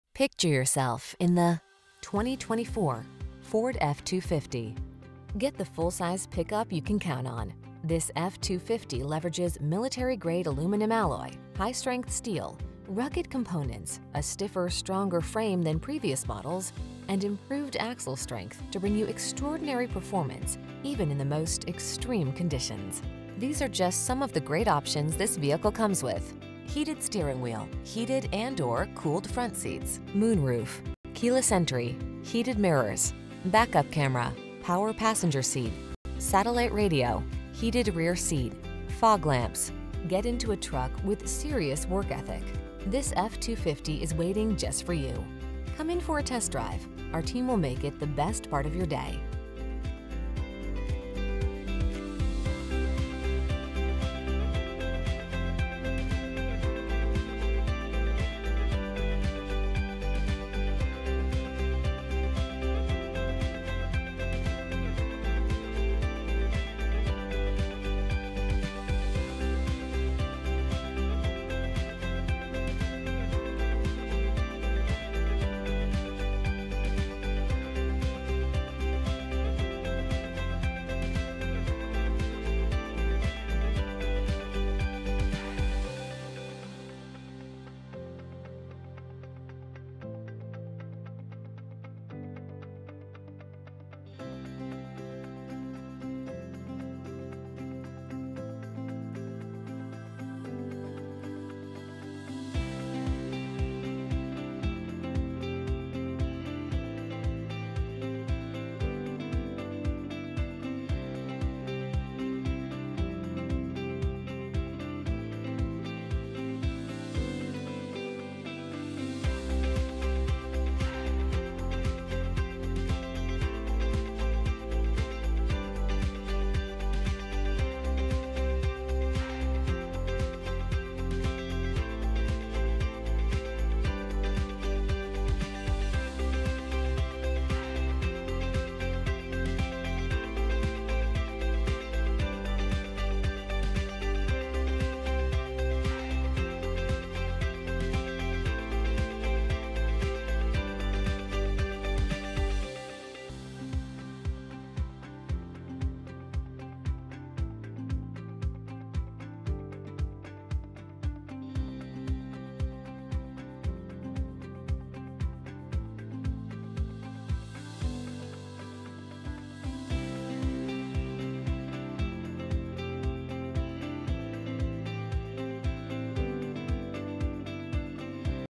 2024 Ford F-250SD Platinum CA Palm Springs, Indio, La Quinta, Cathedral City, Palm Desert
New 2024 Ford F-250SD Platinum available at Palm Springs Motors located in 69200 E Palm Canyon Dr, Cathedral City, CA, 92234 and servicing the Palm Springs, Indio, La Quinta, Cathedral City, Palm Desert area.
Please mention the stock 24TS16 when you contact the dealership for this vehicle.